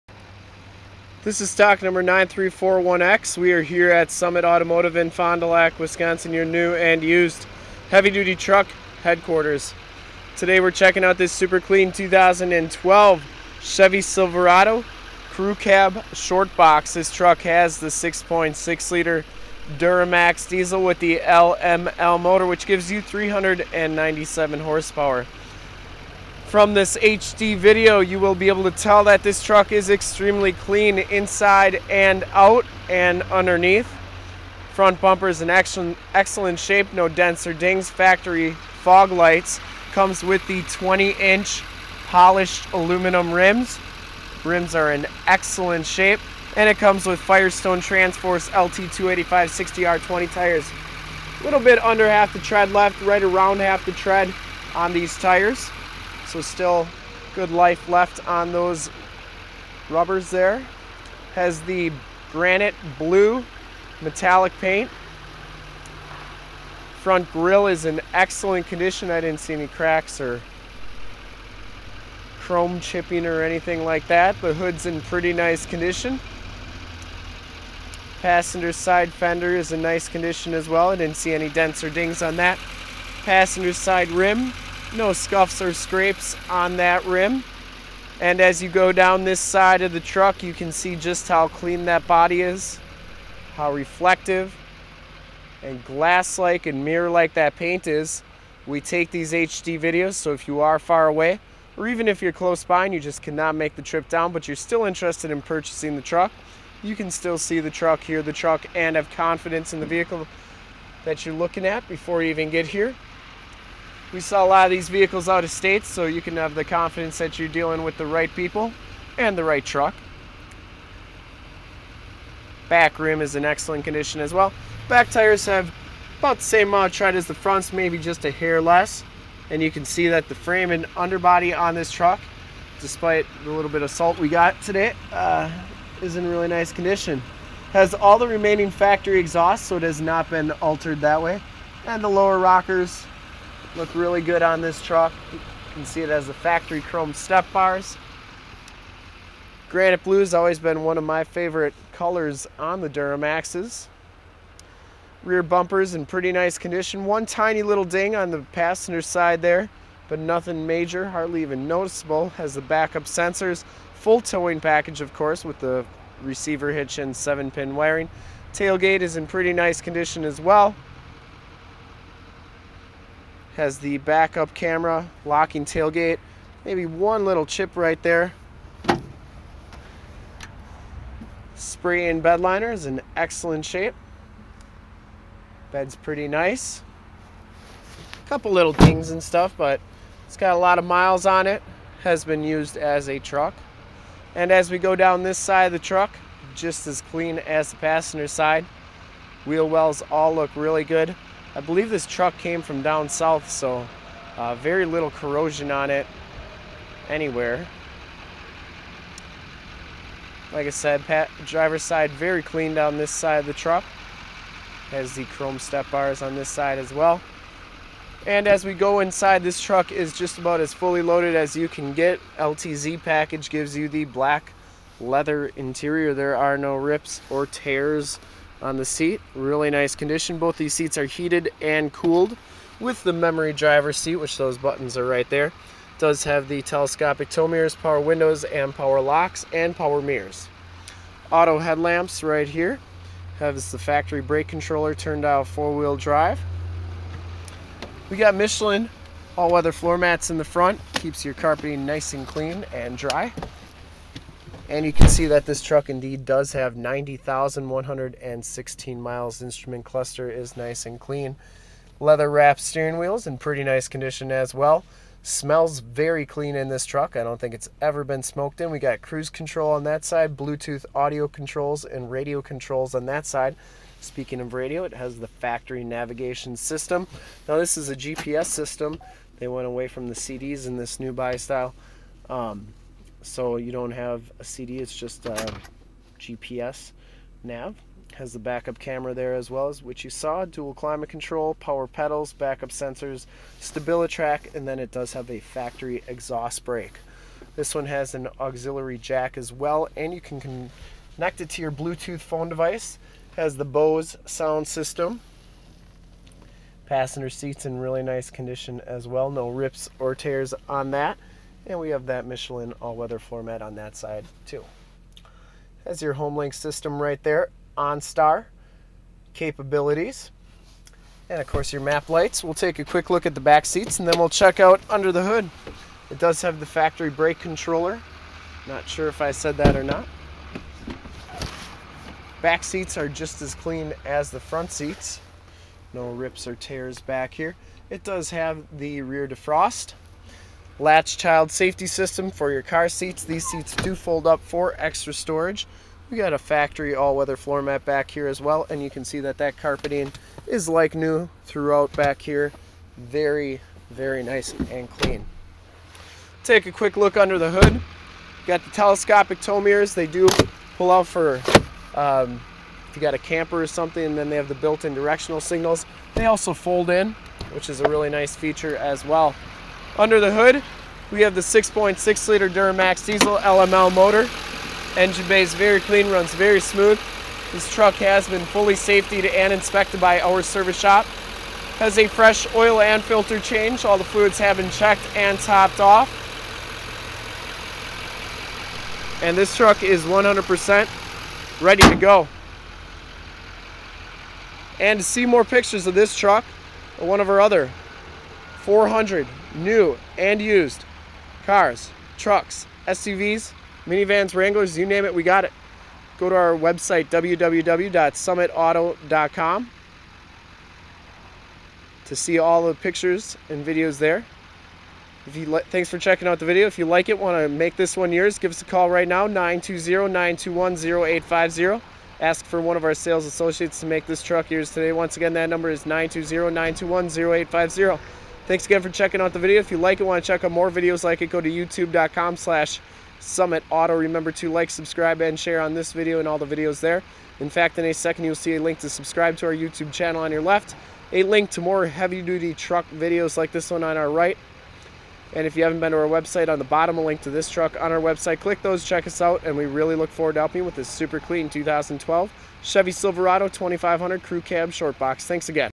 2012 USED CHEVROLET SILVERADO 2500 DURAMAX BLUE GRANITE METALLIC $36,499 SOLD! 9341X
STOCK: 9341X
PRICE: $36,499
MILES: 90,093
MAKE: CHEVROLET
MODEL: SILVERADO 2500
VIN: 1GC1KYE86CF243454

1 Owner! 6.6 Liter V8 Duramax Diesel, LML Engine with 397 Horsepower, Full Four Door Crew Cab, Short Box 6 1/2 Foot Shortbox, LTZ Package 1LZ, 6 Speed Automatic Allison Transmission with Optional Manual Tap Shift, Turn Dial 4x4 Four Wheel Drive 4WD, Z71 Offroad Suspension Package Z-71, Factory GPS Navigation System, Reverse Backup Camera Rearview Camera, Onstar System, Non Smoker, Dual Power Air Conditioned Ventilated and Heated Seats, Black Ebony Leather Seats, Bucket Seats, Memory Driver's Seat, Full Towing Package with Receiver Trailer Hitch, Wiring and Transmission Cooler Tow Package, Factory Brake Controller, Factory Exhaust Brake, Telescopic Tow Power Mirrors with Built-in Directional Signals, Stabilitrak Traction Control, 3.73 Gears with Automatic Locking Differential Limited Slip Differential, 20 Inch Rims Premium Wheels, Firestone TransForce AT LT285/60 R20 Tires, Polished Aluminum Rims Premium Wheels, Four Wheel Disc Brakes, Factory Chromed Stepbars, Spray-in Bedliner, Fog Lights, Reverse Sensors, EZ Raise Assist Tailgate, Locking Tailgate, AM / FM Radio Tuner, Sirius/XM Satellite Radio Capabilities Sirius / XM, CD Player, Bose Premium Audio Sound System, Bluetooth, Hands-Free Phone Controls Blue Tooth, Auxiliary MP3 Jack Portable Audio Connection, USB Jack Portable Audio Connection, Keyless Entry with Factory Remote Start, Rear Window Defroster, Adjustable Height Seatbelts, Driver and Passenger Front Air Bags, L.A.T.C.H. Child Safety System, Side Curtain Air Bags SRS Safety Restraint System, Multi-Function Steering Wheel Controls, Homelink System with Three Programmable Buttons for Garage Doors, Lighting Systems & Security Systems, Compass, Outside Temperature Display and Mileage Display, Dual Multi-Zone Climate Control , Power Adjustable Pedals, Air Conditioning AC, Cruise Control, Power Locks, Power Windows, Tilt Steering Wheel, Automatic Headlights Autolamp, Granite Blue Metallic, One Owner! Very very clean inside and out! This is one of the sharpest 2012 Chevrolet Silverado 2500 crewcab shortbox 3/4 ton diesel trucks on our lot! Make your move before this super clean 4wd is gone!

STOCK: 9341X
PRICE: $36,499
MILES: 90,093
MAKE: CHEVROLET
MODEL: SILVERADO 2500
VIN: 1GC1KYE86CF243454